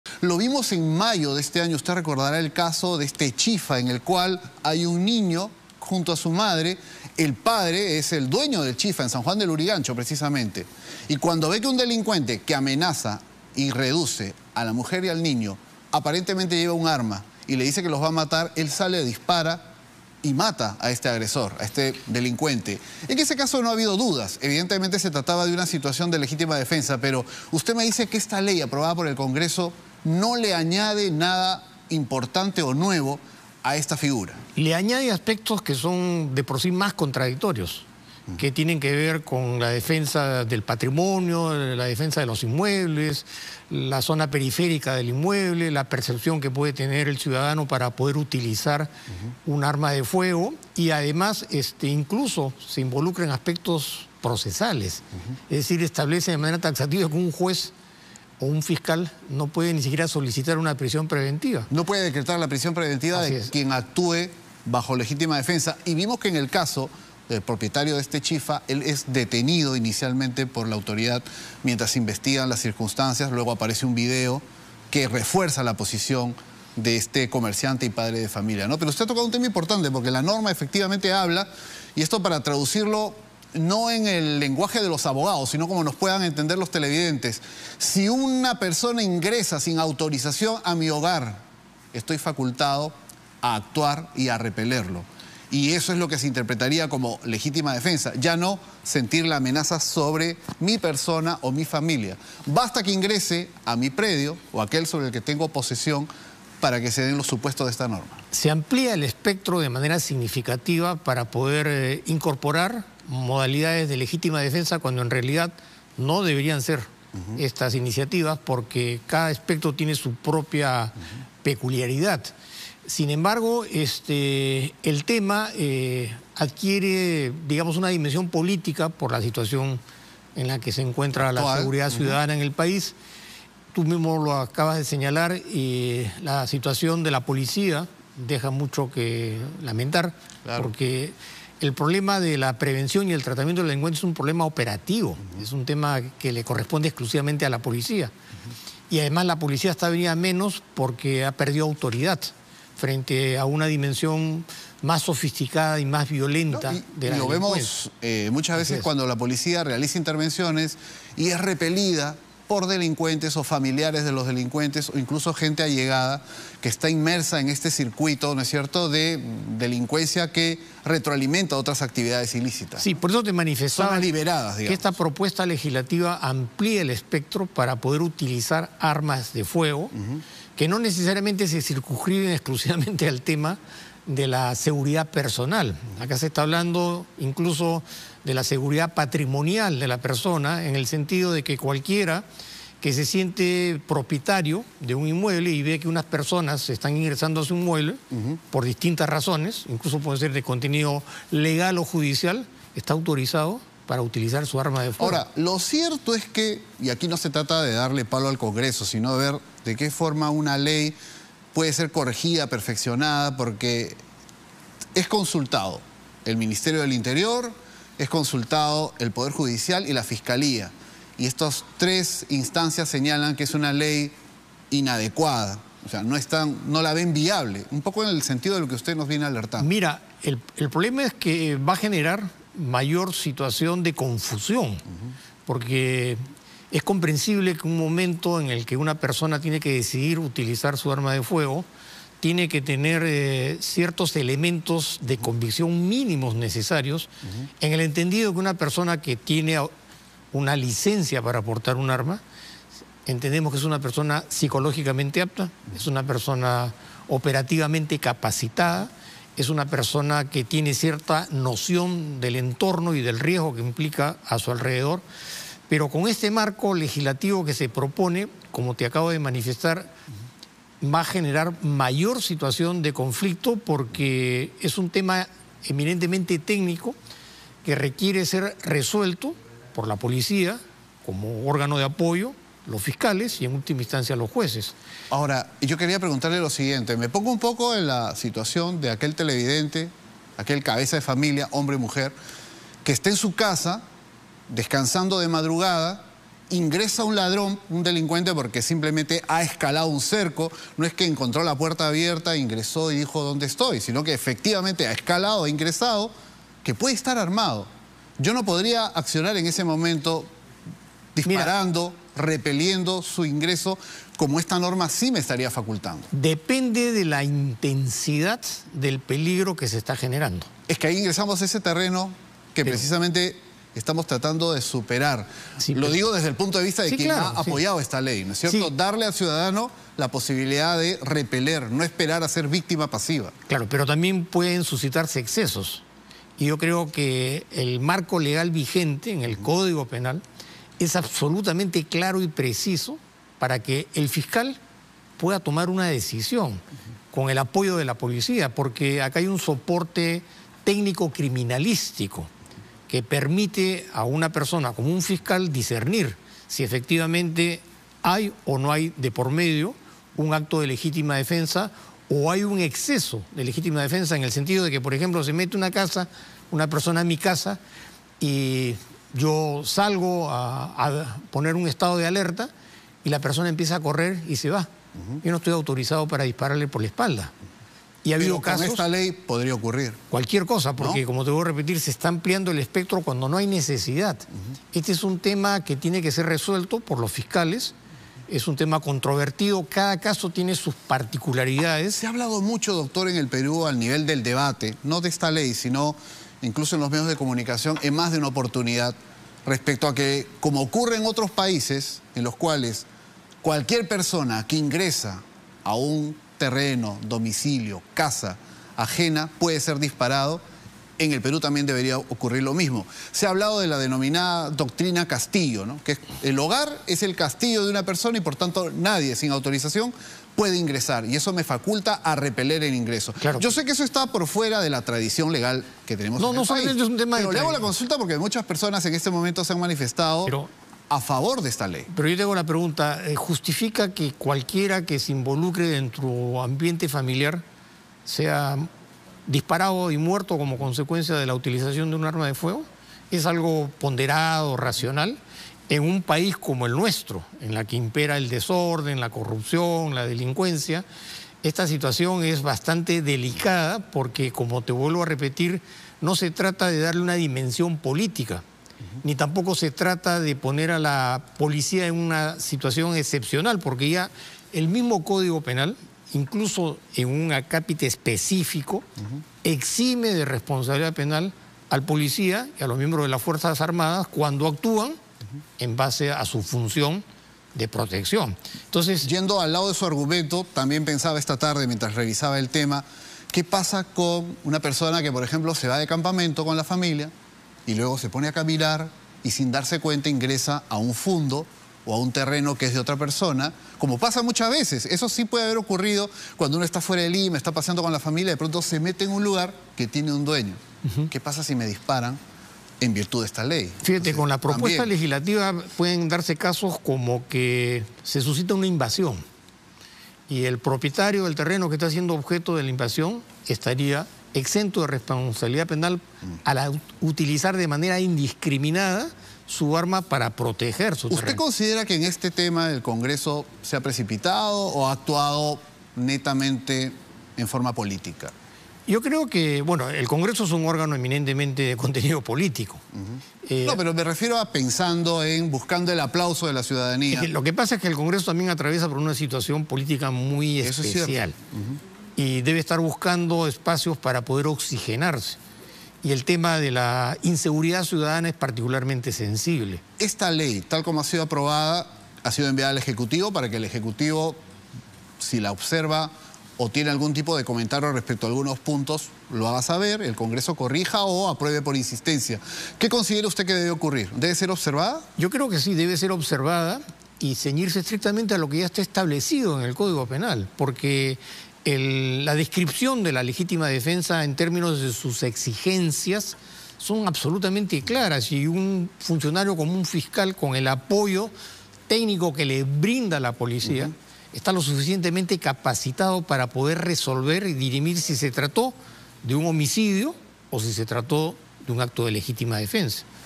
Ley de legítima defensa en Perú: ¿Se debería poder disparar si alguien entra a tu casa?
LatinaNoticias conversó con el abogado Luis Lamas Puccio sobre los cambios que plantea el Congreso en la ley de legítima defensa. Estos fueron los cuestionamientos que hizo al respecto.

Las noticias de hoy, en videos y transmisiones en vivo (streaming). Contenido verificado y riguroso, interesante e importante.